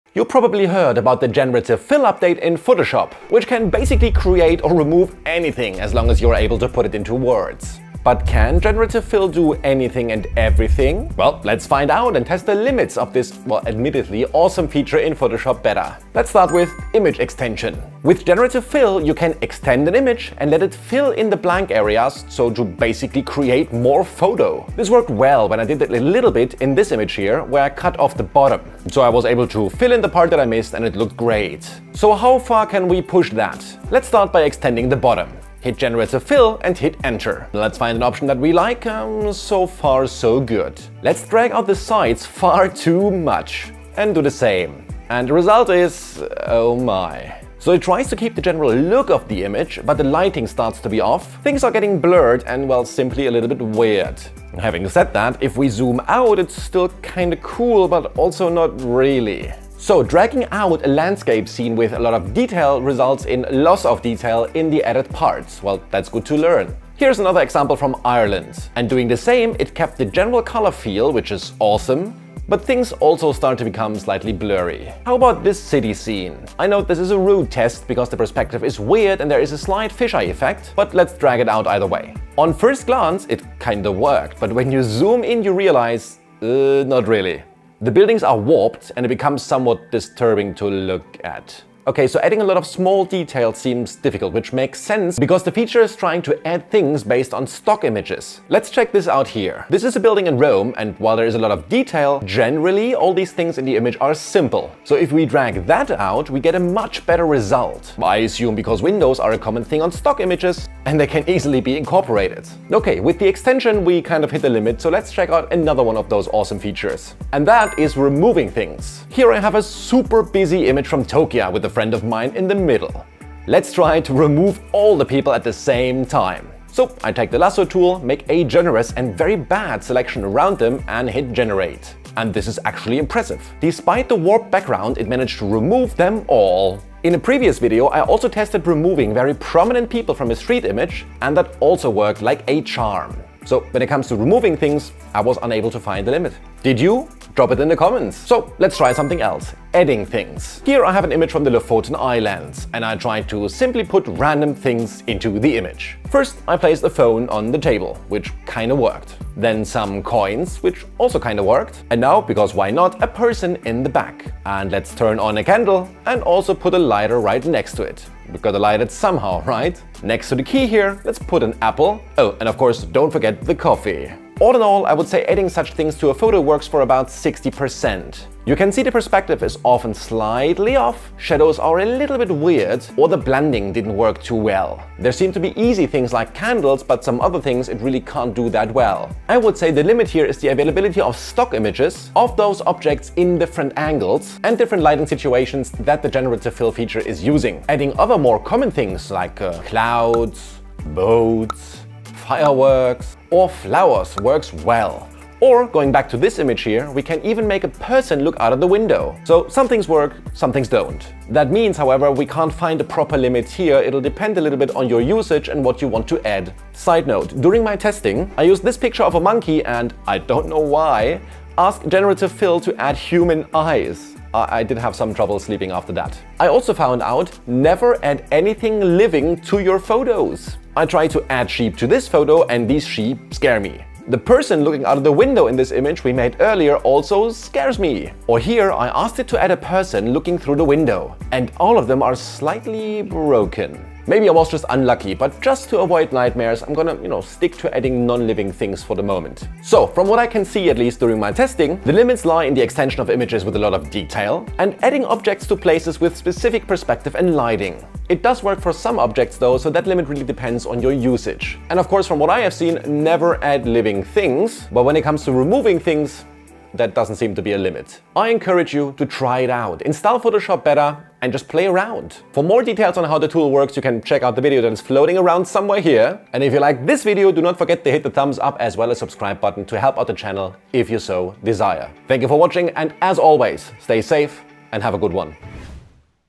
Testing The LIMITS Of Photoshops Generative Fill Feature!
Testing The Limits of Photoshops Generative Fill Feature! - Discover the limits of the generative fill feature in Photoshop Beta! In this video, we put the feature to the test to see if it can truly do anything and everything. From extending images to removing and adding objects, we explore the capabilities of generative fill - properly.

🖼️ Image Extension: Watch as we extend images to fill in blank areas and ... create more photo. We push the feature to its limits, examining the impact on lighting, blurring, and details in landscape, city, and architectural scenes.

🌉 Removing Things: We showcase the impressive ability of generative fill to remove unwanted objects from busy images, testing its limits and inviting viewers to share their experiences in the comments.

➕ Adding Things: Join us as we add various objects to photos, evaluating the success rate and discussing limitations when it comes to perspective, shadows, and blending. We learn the importance of using non-living objects to avoid unexpected and eerie results.

🧩 The Limits: We discuss the limits of generative fill, highlighting challenges with image extensions that have intricate details and objects that require specific perspective and lighting. We also stress the importance of avoiding the addition of living things for more pleasant editing experiences.

Ready to test generative fill yourself? Install Photoshop Beta and start exploring the endless creative possibilities! Stay safe and have a great time experimenting!

Timestamps:
00:00 Intro
00:15 Image Extension
01:50 Removing Things
02:25 Adding Things
03:30 Quick Shock
03:50 Living Things

My Gear:
Camera: Sony a7III
Amazing Wide Angle Lens (Tamron 2.8/15-30mm)
Great Zoom Lens (Sony 3.5/24-240mm)
Awesome Prime Lens (Sony 1.8/50mm)